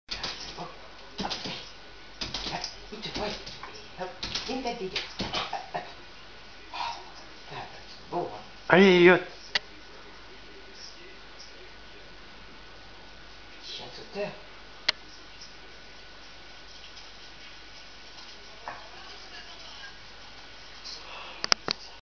Chien sautant devant la TV
Notre chien qui saute devant un film,nous avons trouvé sa très drôle il le fait souvent